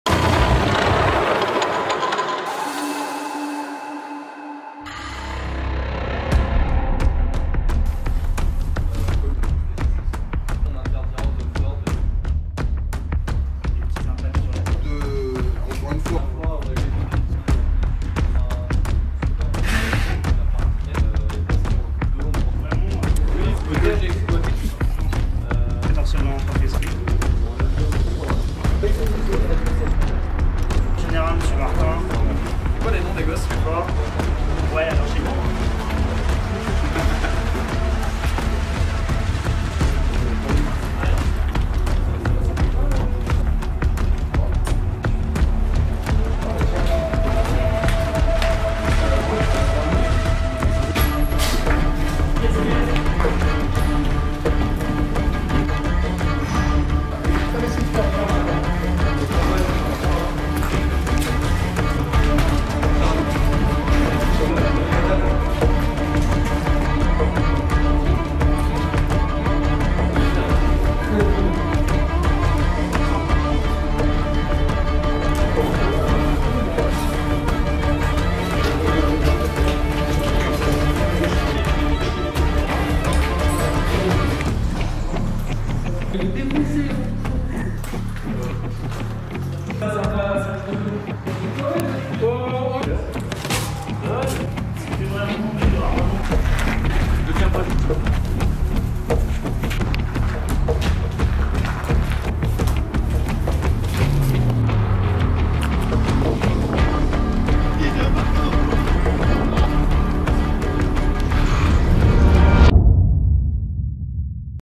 Milsim Airsoft France | DRILL AURA | Groupe DIL | Airsoft CQB
Vidéo d'un drill CMC au profit des équipes auramilsim. 8 équipes au total étaient présentes pour cet évènement, qui fut séparé en 2 phases : le matin fut consacré à des drill, chaque équipe pouvant travailler en autonomie ou participer à un enchaînement de petites MES ; et l’après midi fut l’occasion de réaliser des MES de plus grand envergure avec un effectif conséquent sur l’ensemble du complexe.

CMC drill for the AURA MilSim teams. A total of 8 teams participated in this event, which was divided into two phases: the morning was dedicated to drills, where each team had the opportunity to work autonomously or participate in a series of small scenario-based exercises. In the afternoon, we conducted larger-scale scenario-based exercises with a significant number of participants across the entire complex.